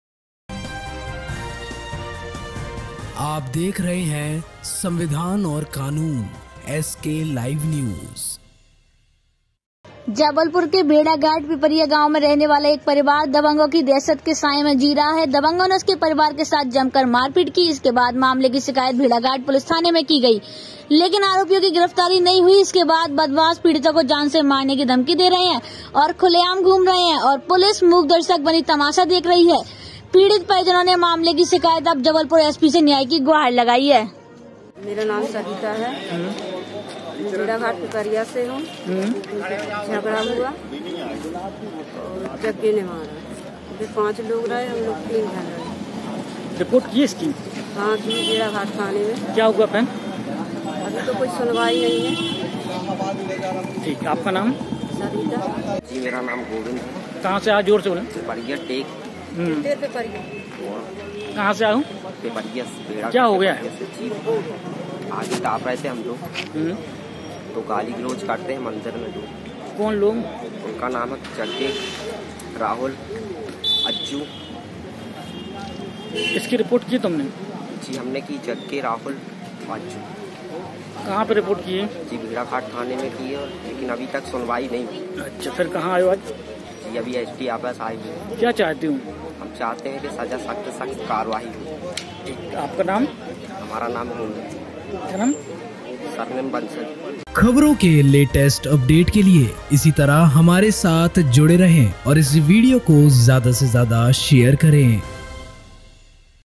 जबलपुर के भेड़ाघाट पिपरिया गांव में एक परिवार दबंगों की दहशत में
जबलपुर के भेड़ाघाट पिपरिया गांव में रहने वाला एक परिवार दबंगों की दहशत के साए में जी रहा है दबंगों ने उसके परिवार के साथ जमकर मारपीट की इसके बाद मामले की शिकायत भेड़ाघाट पुलिस थाने में की गई लेकिन आरोपियों की गिरफ्तारी नहीं हुई इसके बाद बदमाश पीड़ितों को जान से मारने की धमकी दे रहे हैं और खुलेआम घूम रहे हैं और पुलिस मूकदर्शक बनी तमाशा देख रही है पीड़ित परिजनों ने मामले की शिकायत अब जबलपुर एसपी से न्याय की गुहार लगाई है